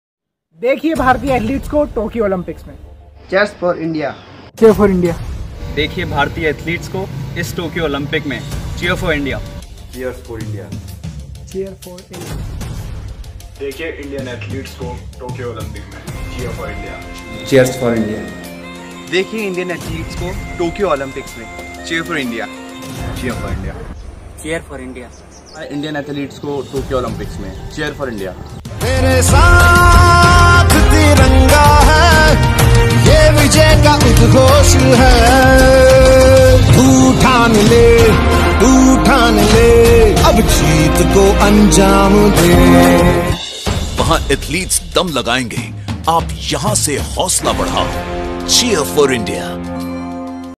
NSS Volunteer cheering to Indian Athletes in Tokyo Olympics
*India is sending strong contingent of more than 100 athletes to Tokyo Olympics.*

So now it's time for us to cheer them